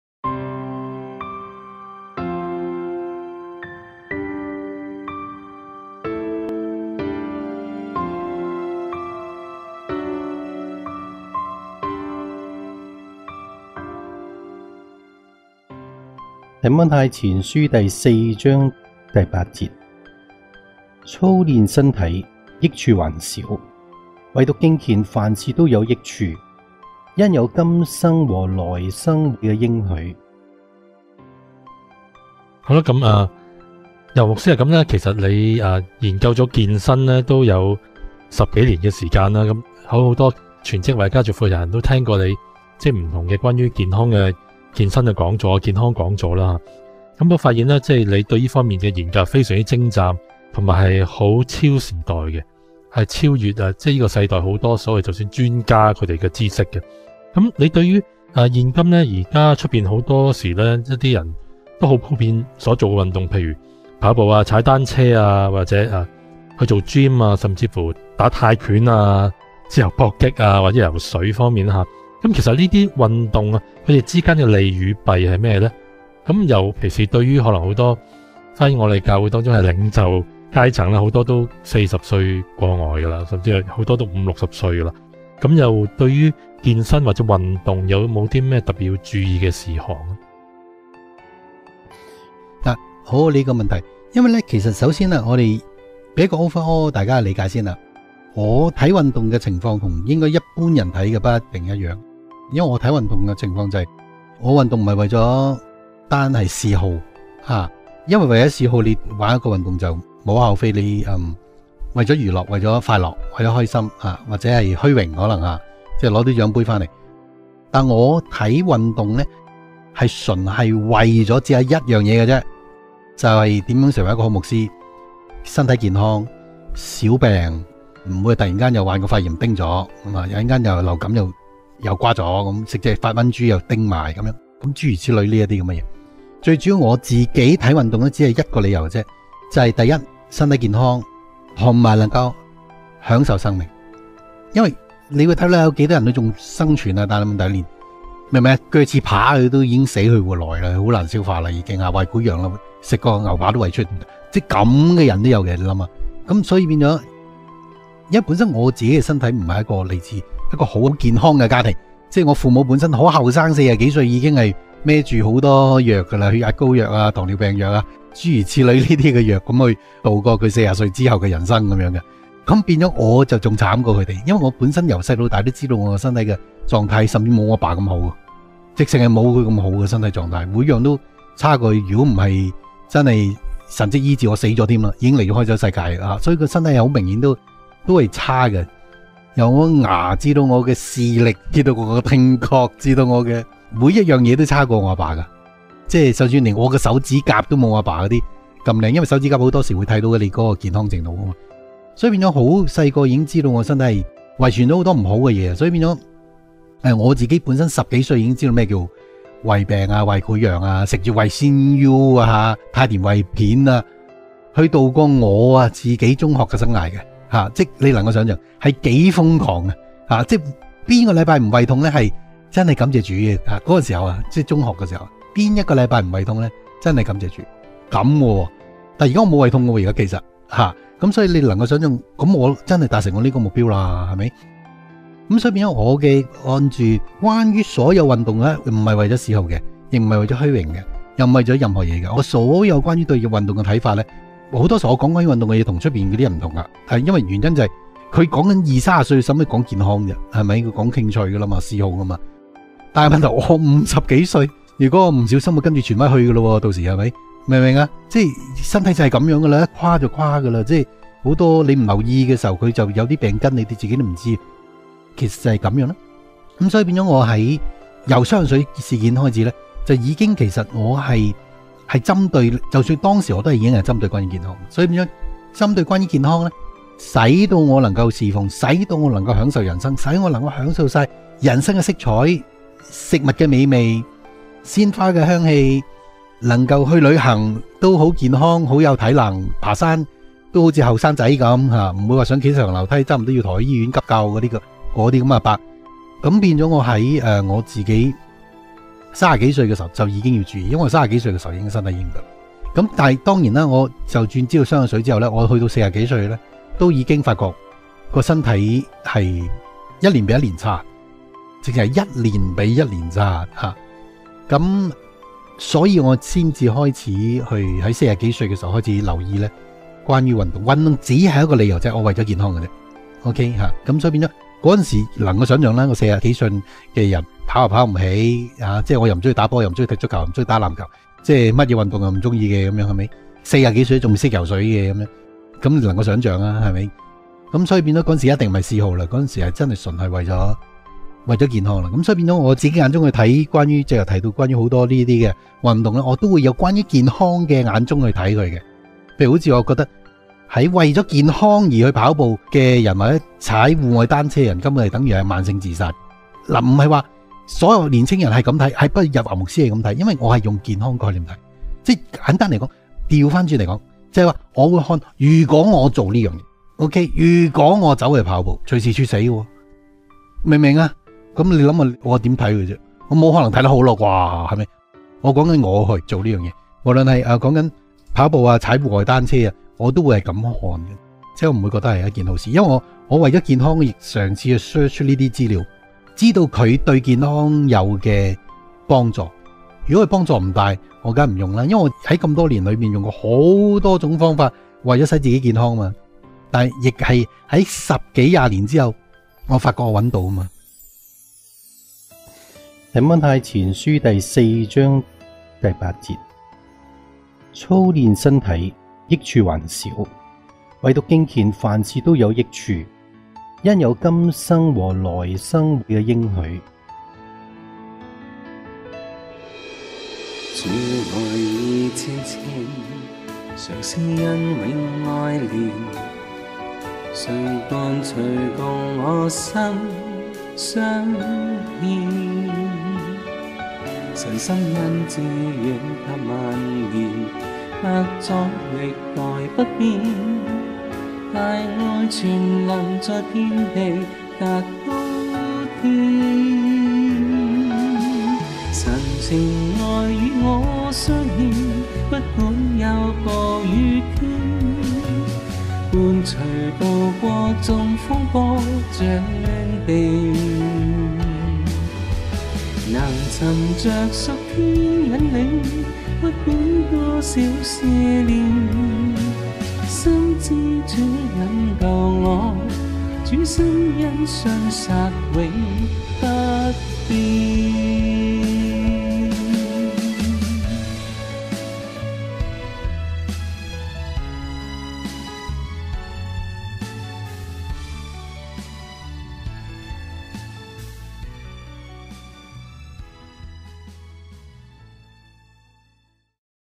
【栽在溪水旁】《提摩太前書4章8節》｜每天靈修系列｜2020-04-14
問題：「日華牧師，你鑽研健身已十多年，許多全職事奉者或家聚負責人都聽過你主講的健身講座，或不同有關健康的講座。

你對這方面研究深入，擁有非常精湛和超時代的見解，無疑跨越了這世代中許多所謂專家的知識。

現時，普遍人做的運動有跑步、踏單車、健身（gym），甚至打泰拳、自由搏擊或游泳，請問這些運動的利與弊是甚麼呢？

尤其對於我們教會的領袖階層，許多已年過四十，更甚是五、六十歲，當他們進行健身等運動時，有沒有一些須特別注意的地方呢？」

這是一條好問題。